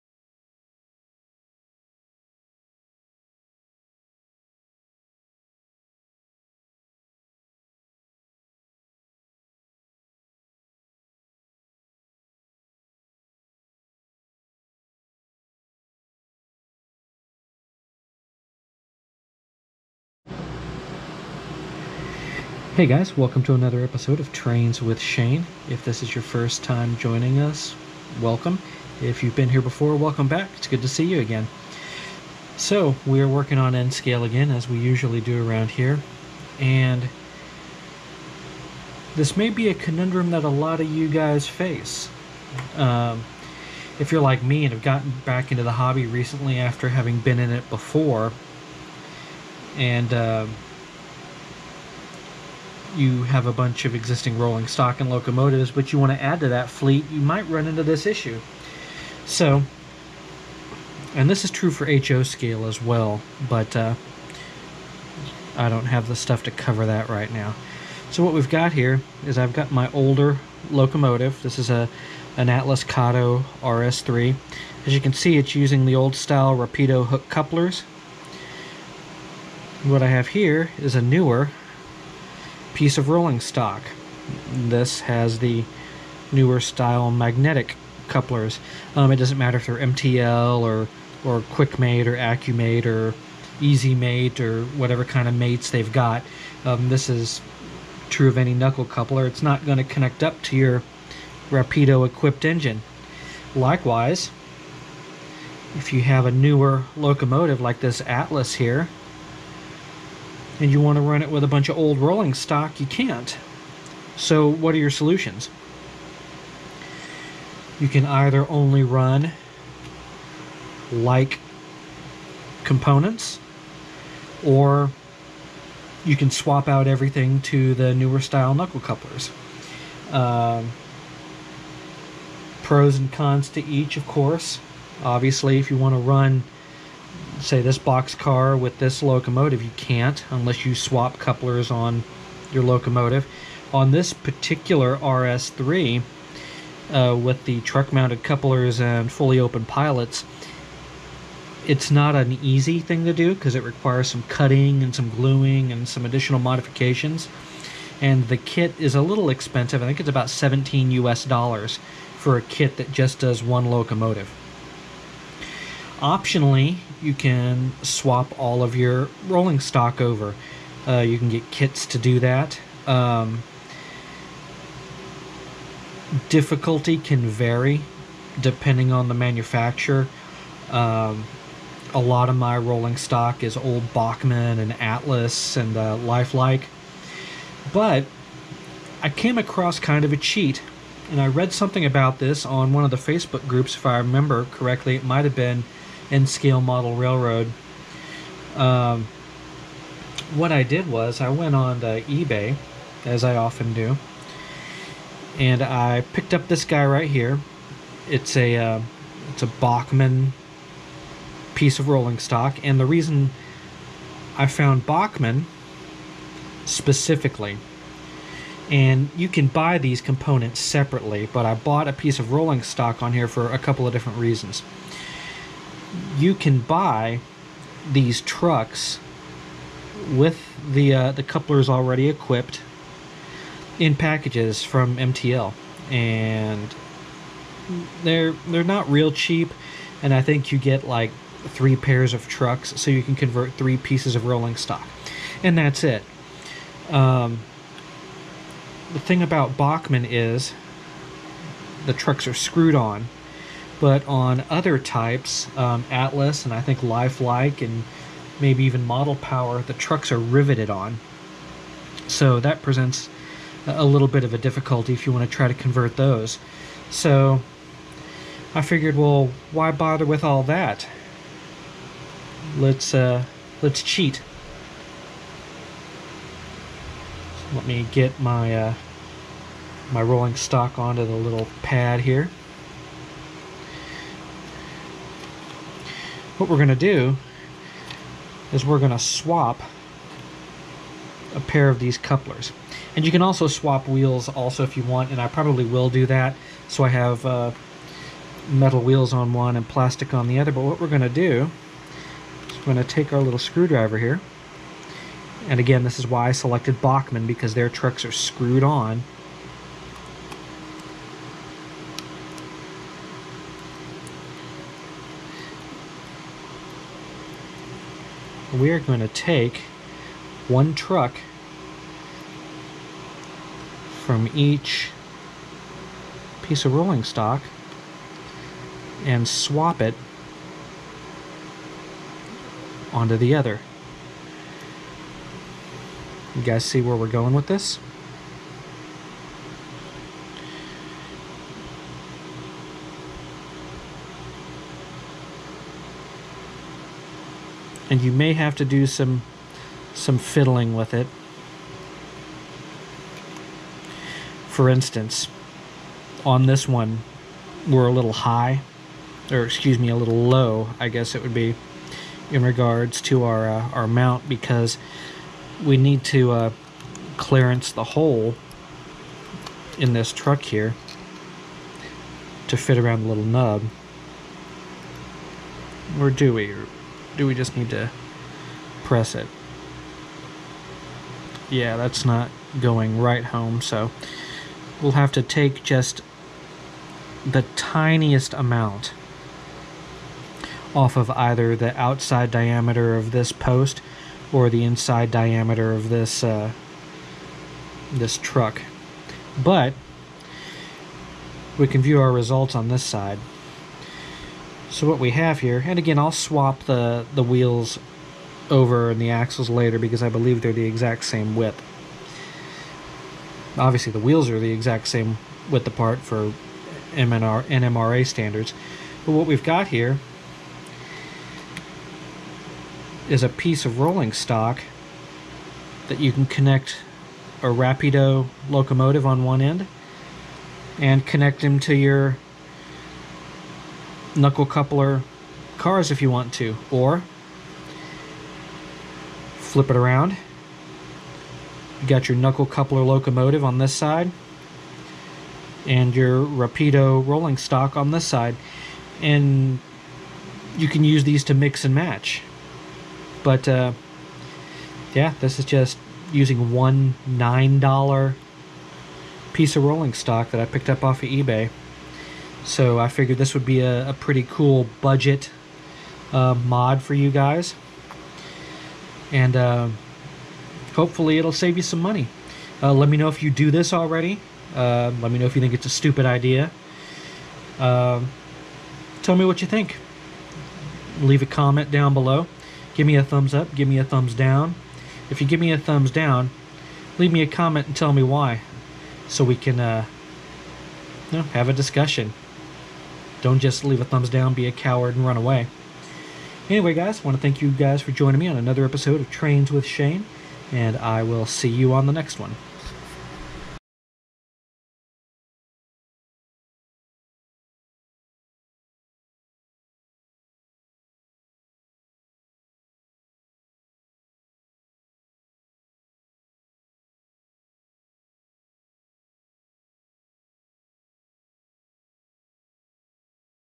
Watch this before spending a bunch of money on replacement couplers for your N Scale rolling stock.
Update: We have Channel Memberships now as well as a Thanks button!

Have a mix of a bunch of old rolling stock with Rapido couplers and newer rolling stock with knuckle style couplers? Not wanting to spend a ton of money to convert the rapido to knuckles? Don't want to spend a ton of money just replacing the rolling stock with newer versions? Well i've got a budget friendly hack that will save you the cost and trouble!